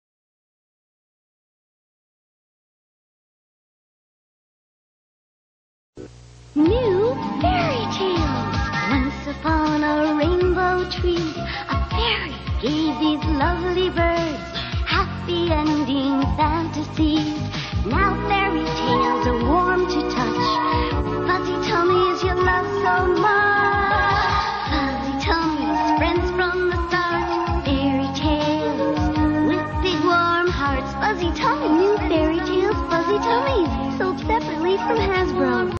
Fairy Tails :: Commercial 2
Fairy Tails were produced by Hasbro in 1986/87. They were a series of colourful plastic bird with long hair and symbols on their chests, similar in colour to My Little Pony.
Each bird came with a perch which their feet could grip onto when they were no longer gripping onto your fingers! They also came with a comb and ribbons for their long multi coloured hair. There were a few playsets produced but I don't know of any other accessories.